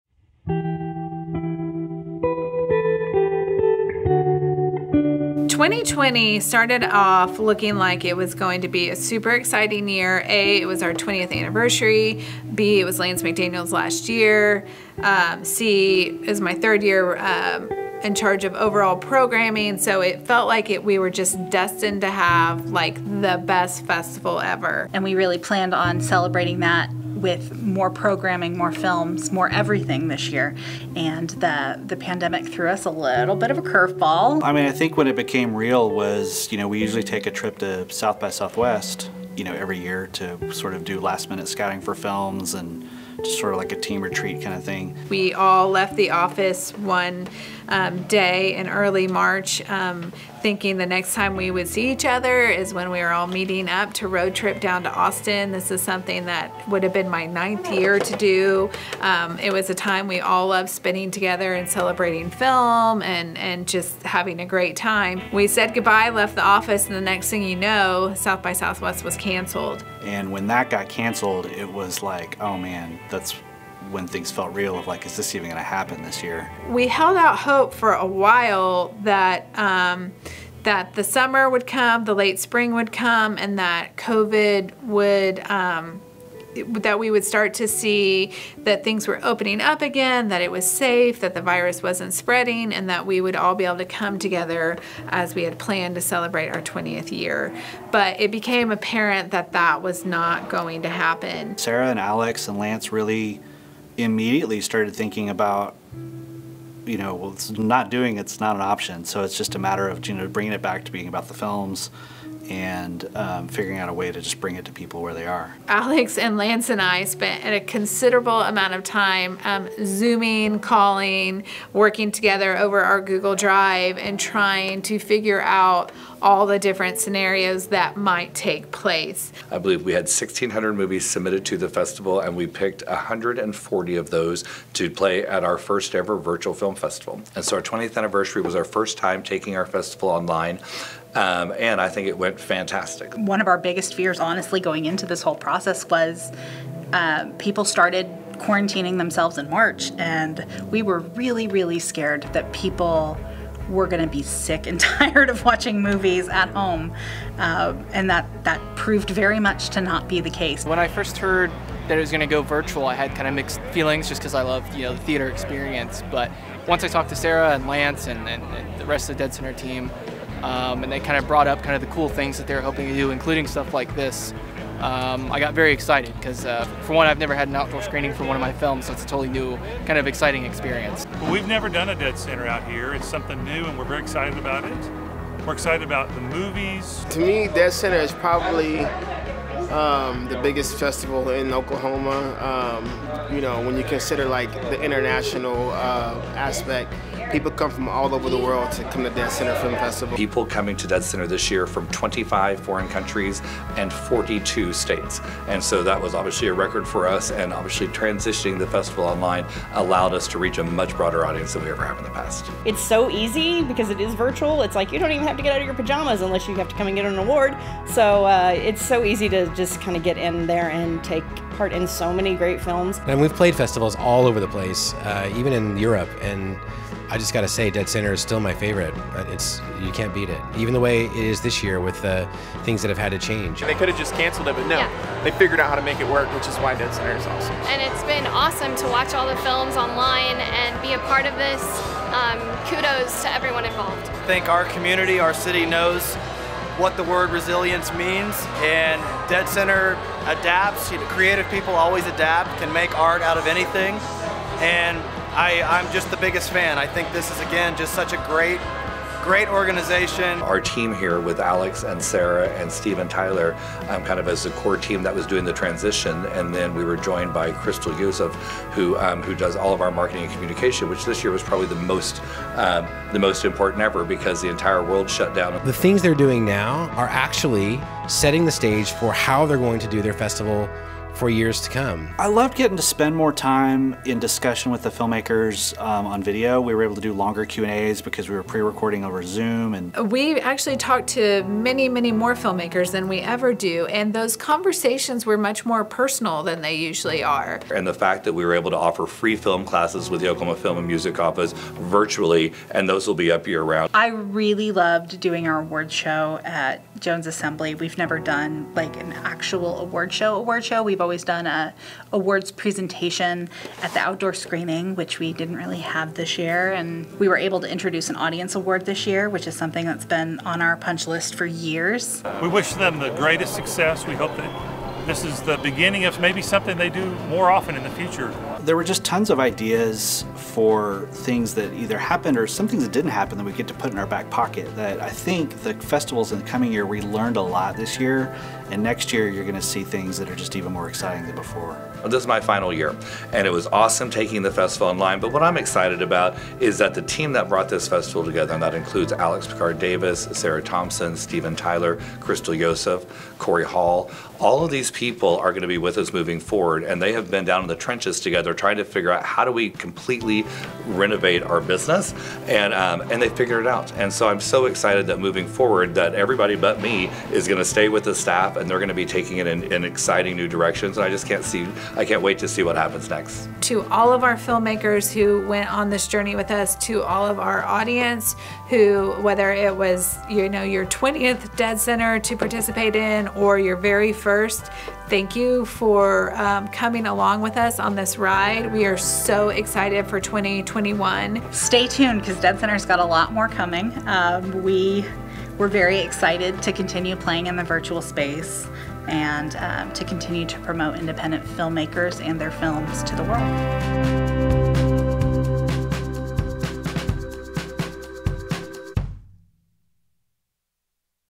deadCenter 20: Pivoting in a Pandemic
In this recap of deadCenter 20, deadCenter staff discuss the challenges, changes, and advantages of pivoting a film festival in the time of COVID-19.